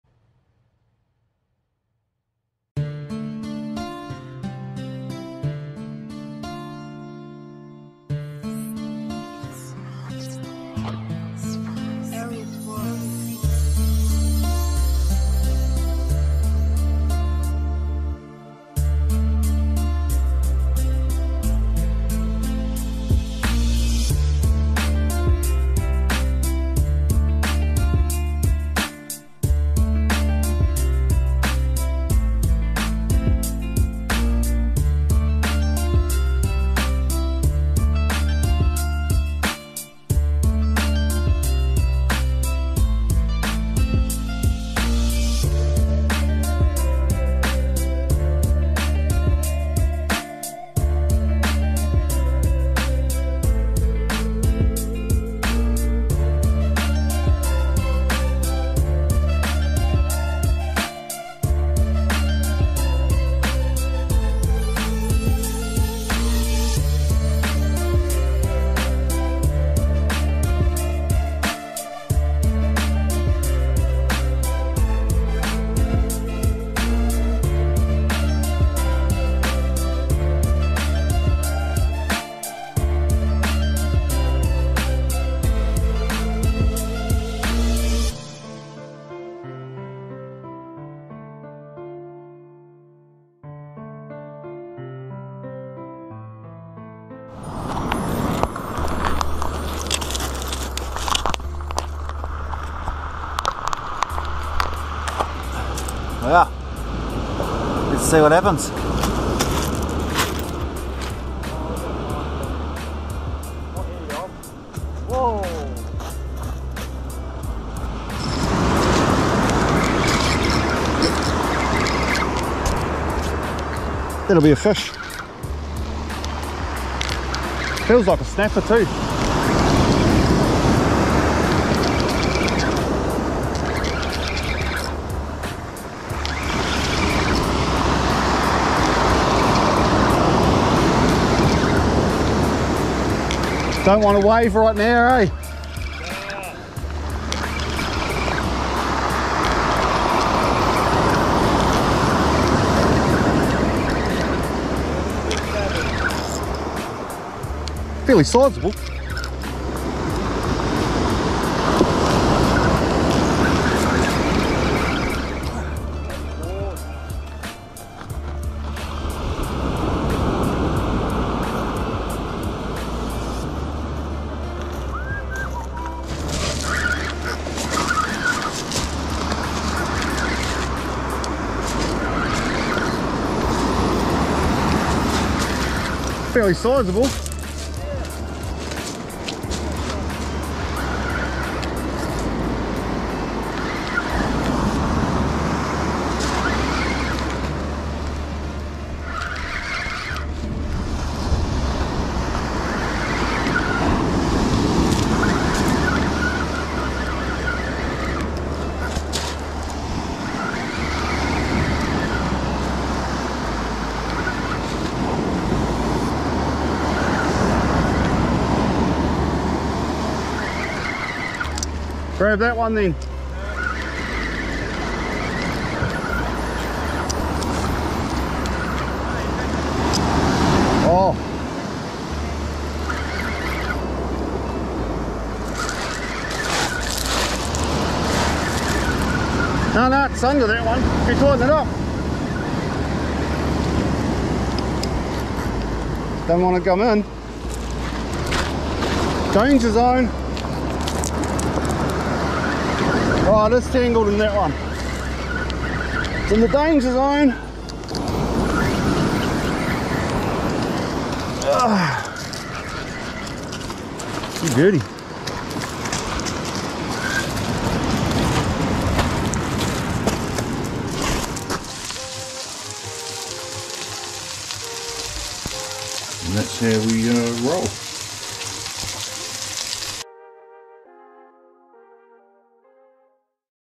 fresh snapper for dinner
wiped down the beach for a quick fish for a cuply snapper to take home for dinner...